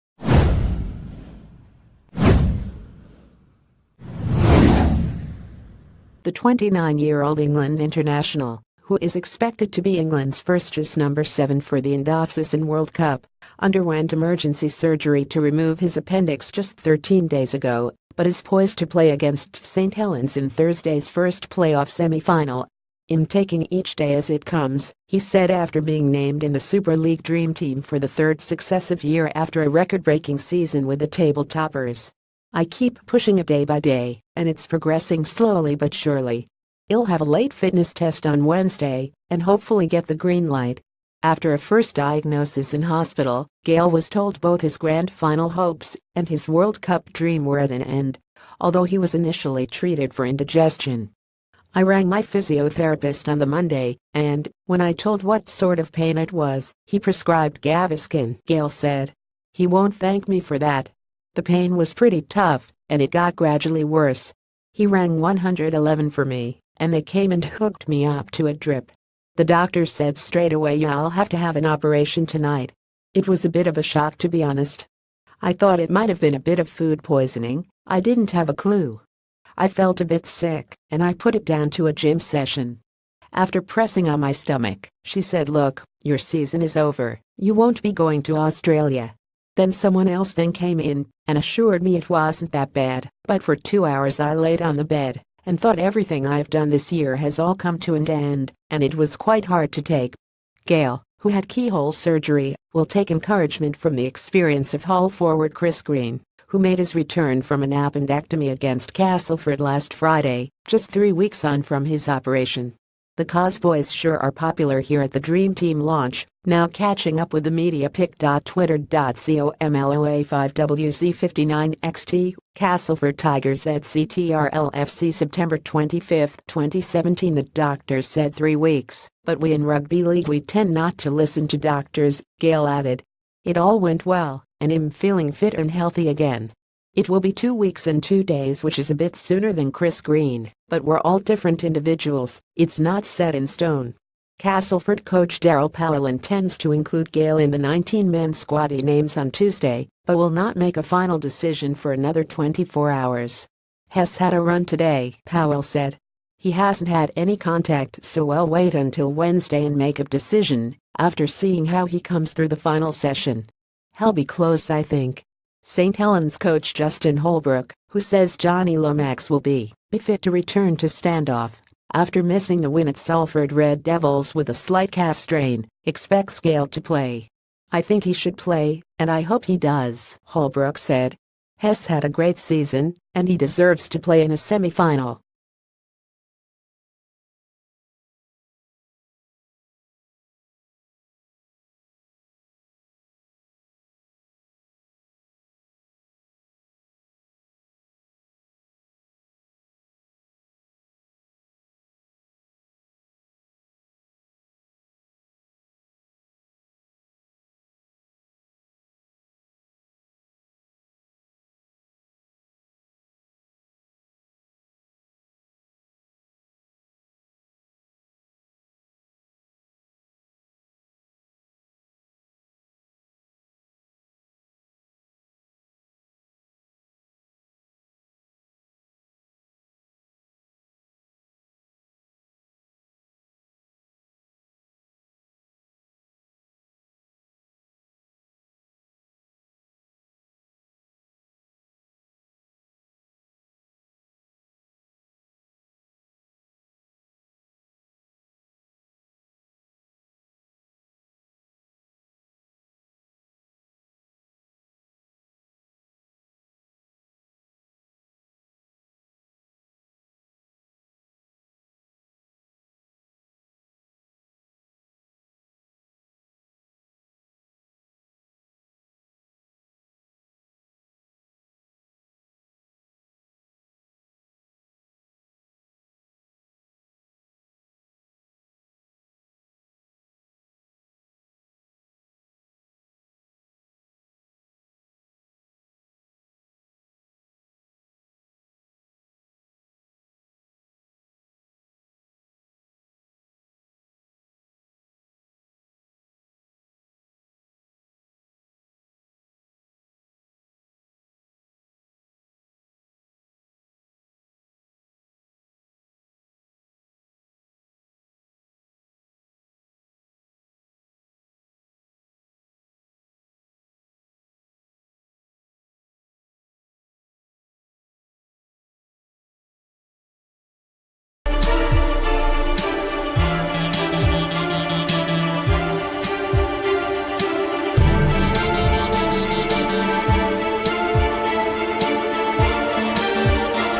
Breaking News | Gale ready to play in semi-final after appendix removal
Gale ready to play in semi-final after appendix removal

The 29yearold England international, who is expected to be Englands firstchoice number seven for the...